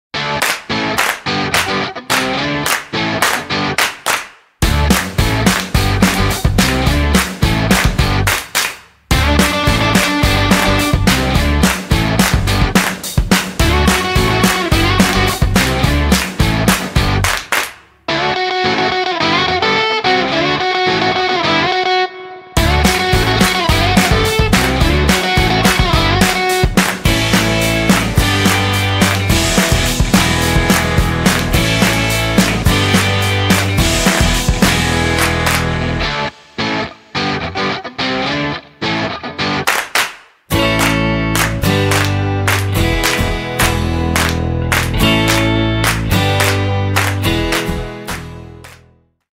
Channel Showreel 2020
A trailer showing my work and the content of my channel. Game jams, programming, video from travels and general geek videos.